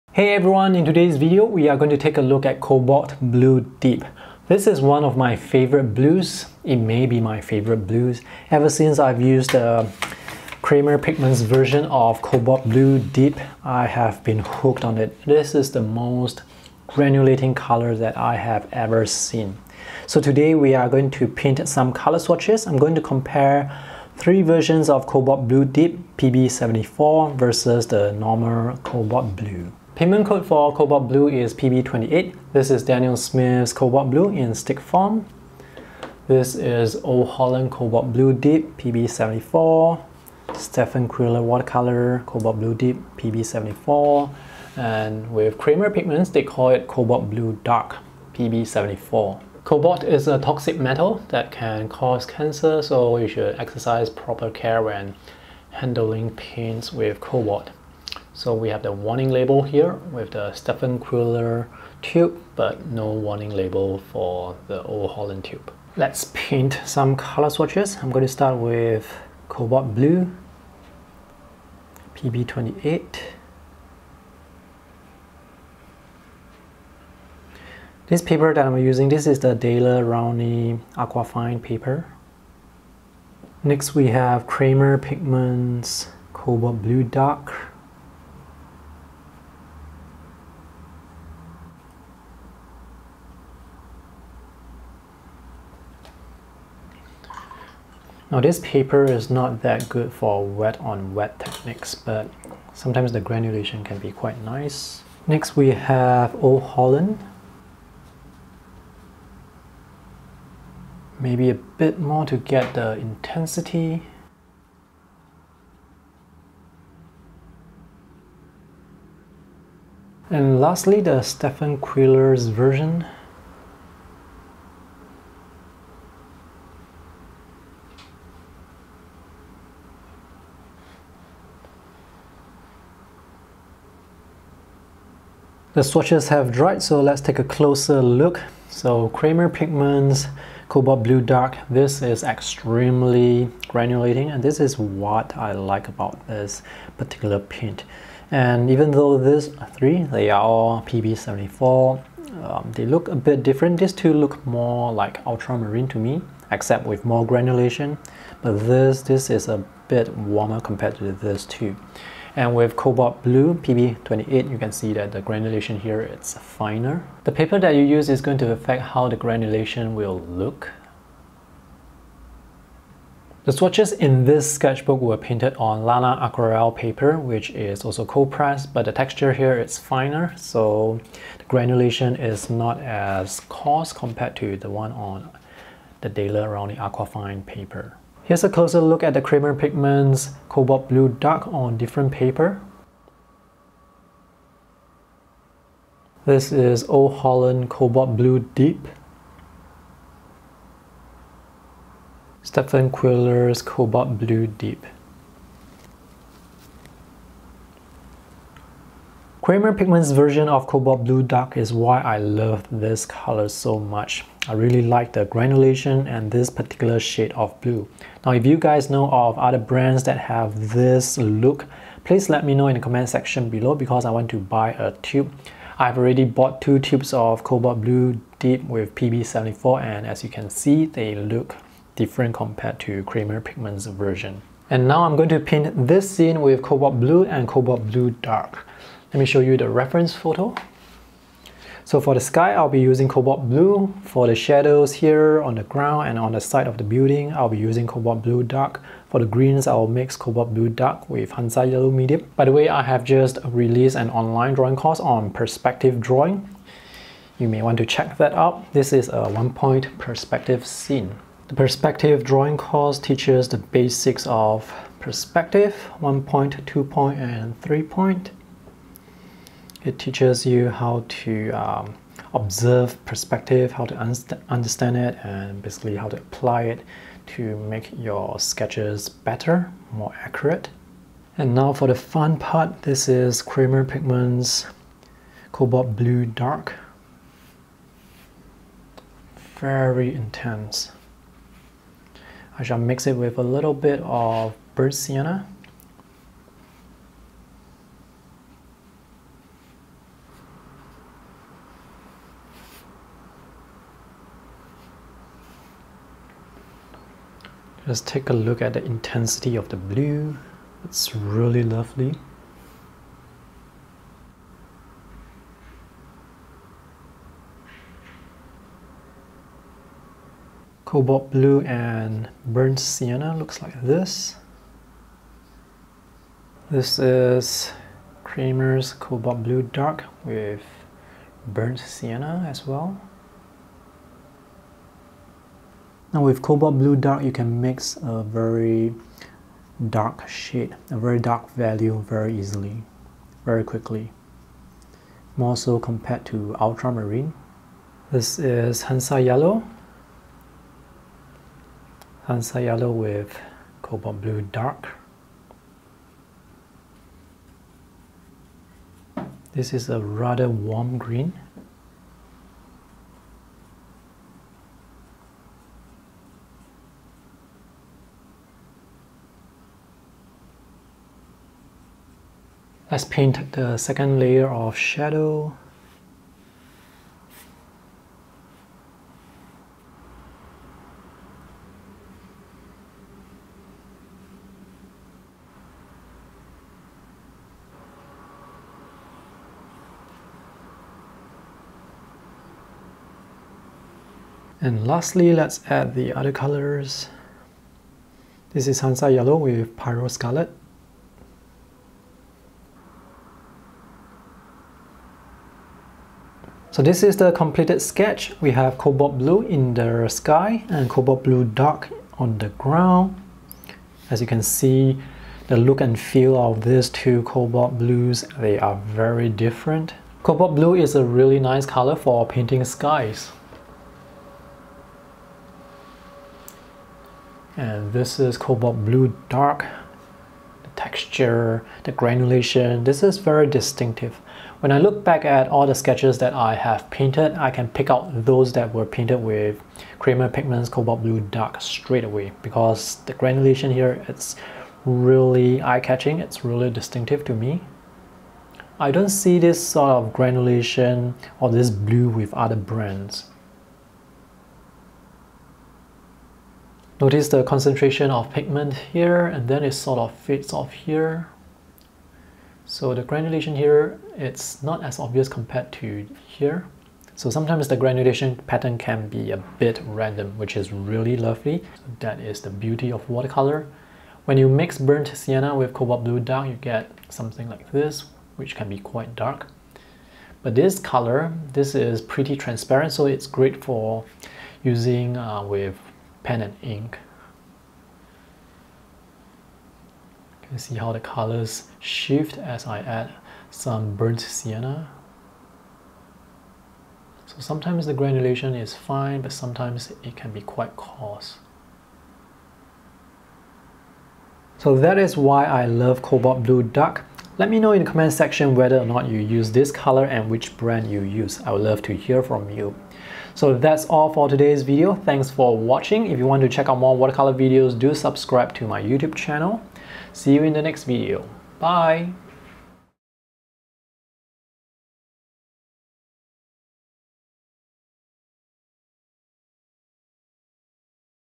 Where to find the perfect Cobalt Blue Deep/Dark (PB74)
I'm trying to find the perfect extra-granulating Cobalt Blue Deep. Kremer Pigment has the best version.